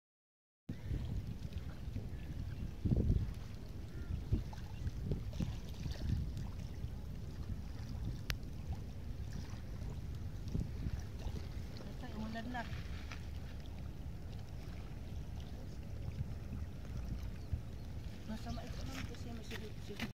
Al Mamzar open beach Dubai UAE new year 2022 couple walk
After a year of pandemic 2020 new year celebration is different from the past years. And it also sad celebrating away from home Philippines. We both prefer to walk and feel the freshness of nature opening our new year.